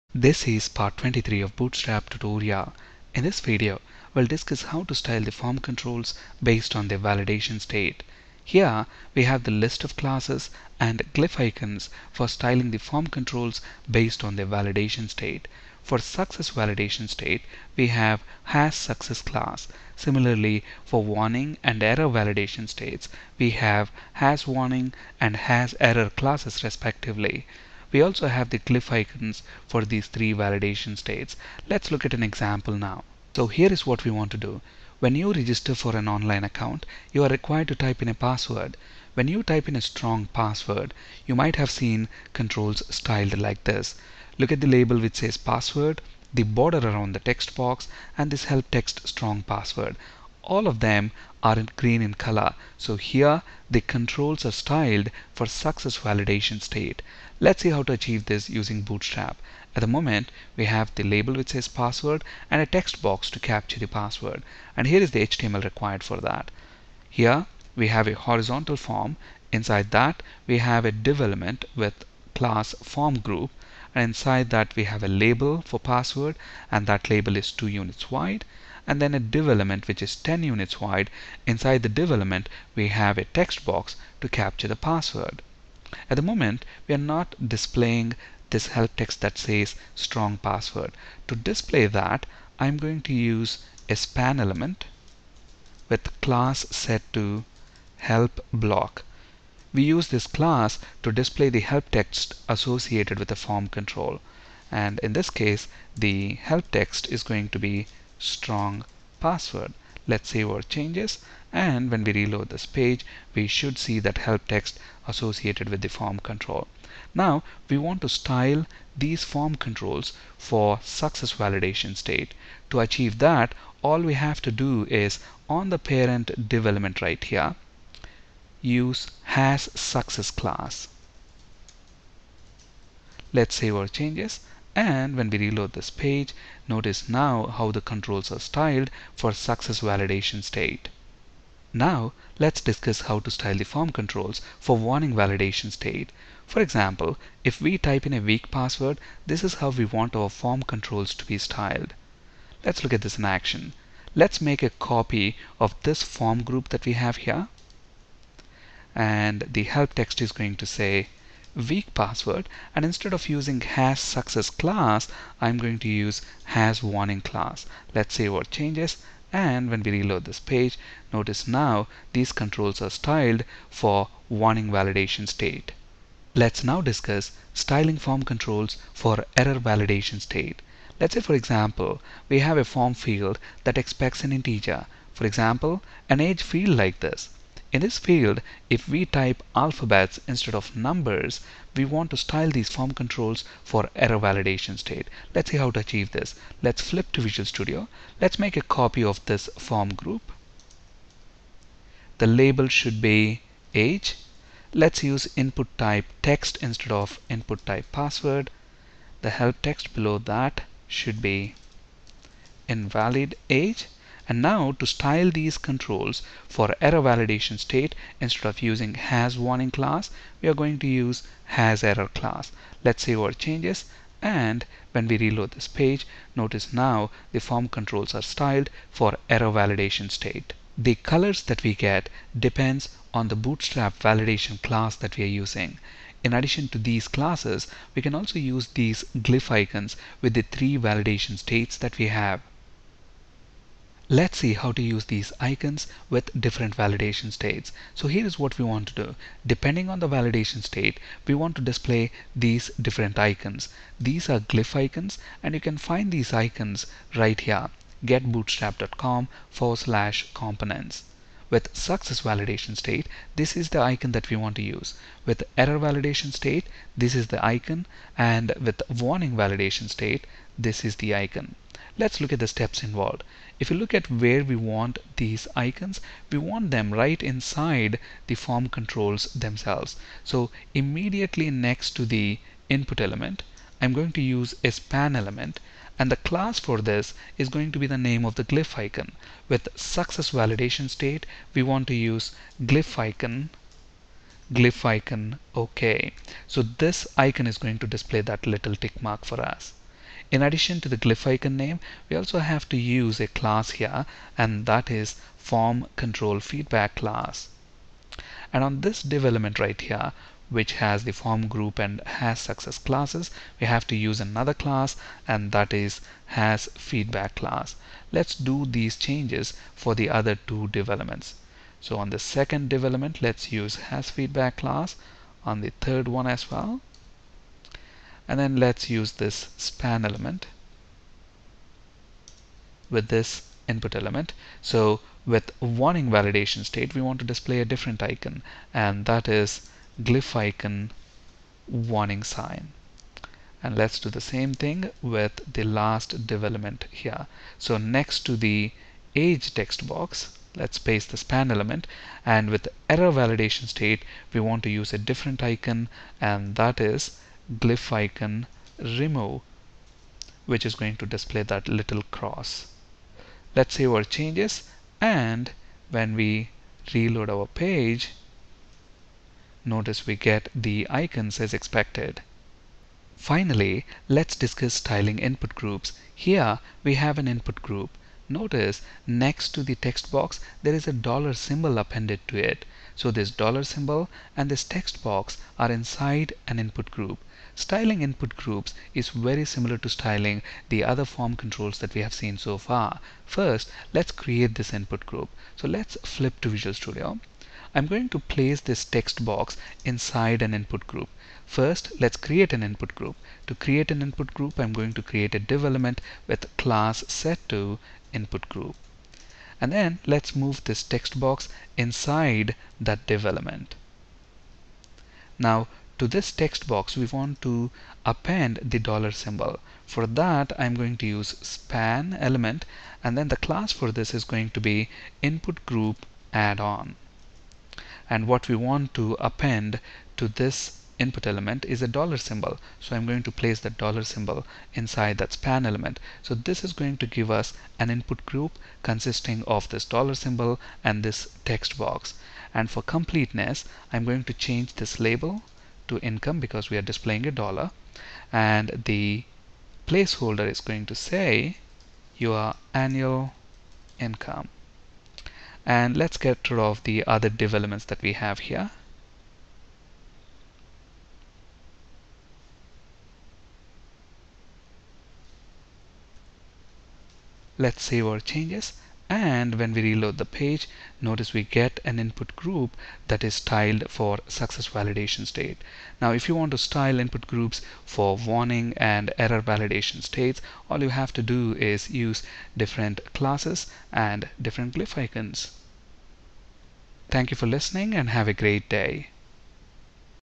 Bootstrap form validation states
In this video, we will discuss the bootstrap classes that can be used to style the form controls based on their validation state.